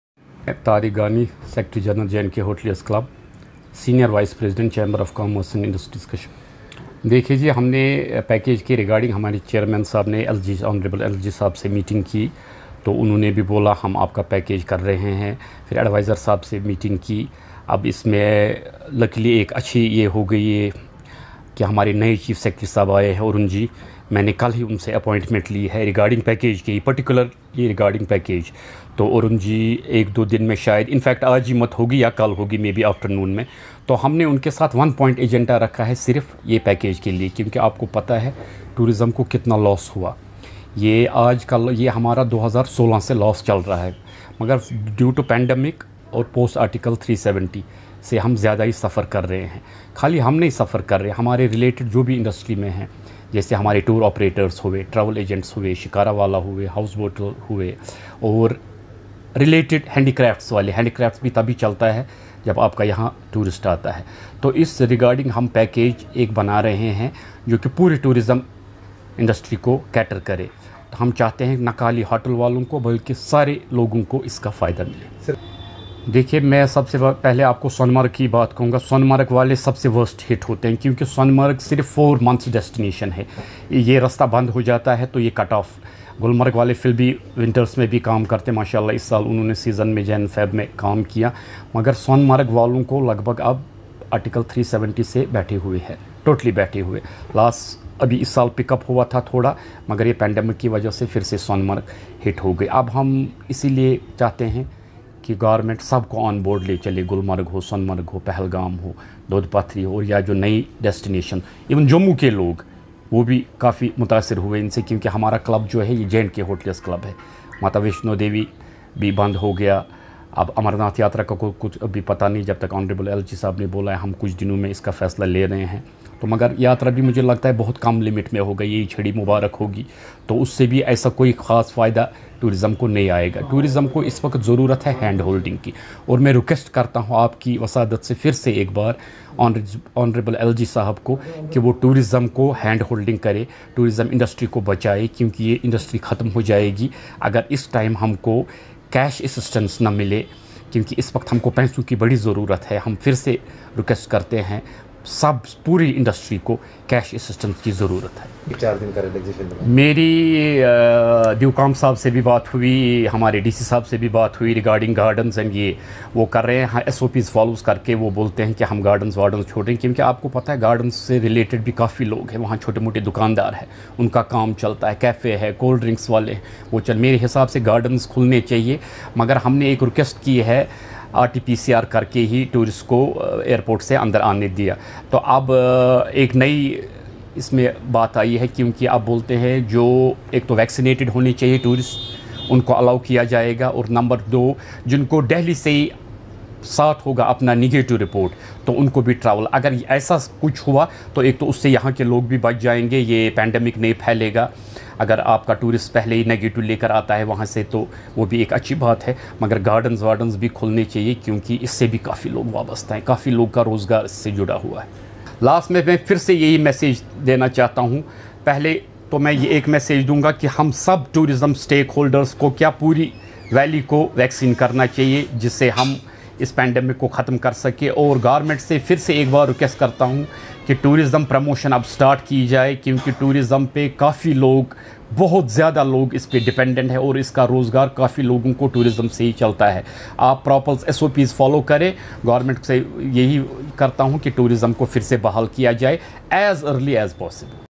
Exclusive Intreaction With General Secretary of Hoteliers Club Mr. Tariq Gaani
Video by Mohsin Altaf